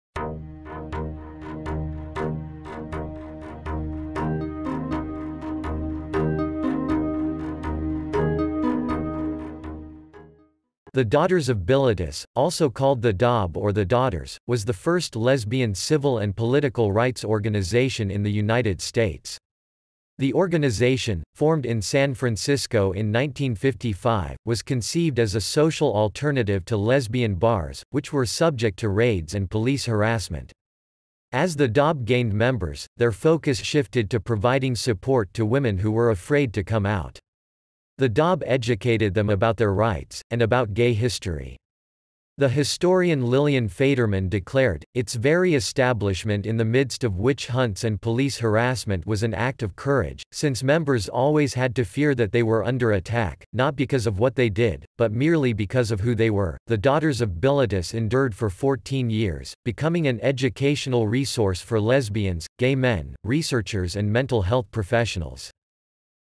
What is Daughters of Bilitis?, Explain Daughters of Bilitis, Define Daughters of Bilitis
~~~ Daughters of Bilitis ~~~

Title: What is Daughters of Bilitis?, Explain Daughters of Bilitis, Define Daughters of Bilitis

Description: The Daughters of Bilitis , also called the DOB or the Daughters, was the first lesbian civil and political rights organization in the United States. The organization, formed in San Francisco in 1955, was conceived as a social alternative to lesbian bars, which were subject to raids and police harassment. As the DOB gained members, their focus shifted to providing support to women who were afraid to come out. The DOB educated them about their rights, and about gay history. The historian Lillian Faderman declared, "Its very establishment in the midst of witch-hunts and police harassment was an act of courage, since members always had to fear that they were under attack, not because of what they did, but merely because of who they were." The Daughters of Bilitis endured for 14 years, becoming an educational resource for lesbians, gay men, researchers and mental health professionals.